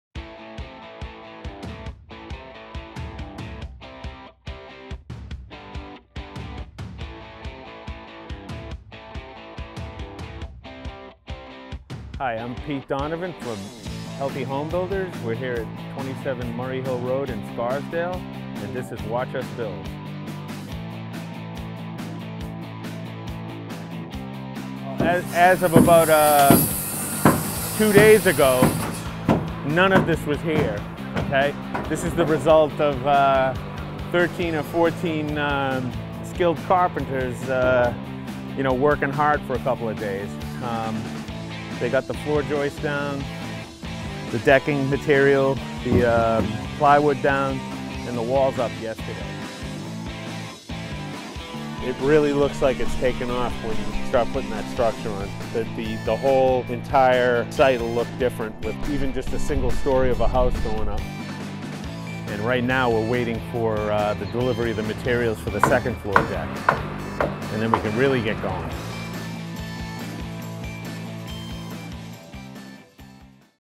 Watch Us Build! Framing at Murray Hill Road
We're framing! The home at 27 Murray Hill Rd in Scarsdale is progressing nicely now that the Spring rains have let up a bit. The General Pete gives us a look at the framing that's making our newest clean air, pure water and green energy project a reality.